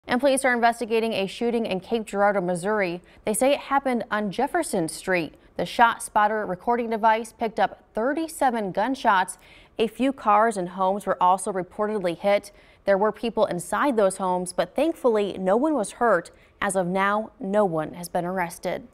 37 shots fired, homes and vehicles hit in Cape Girardeau
Cape Girardeau police are investigating an shooting that took place on Tuesday, resulting in homes and vehicles hit.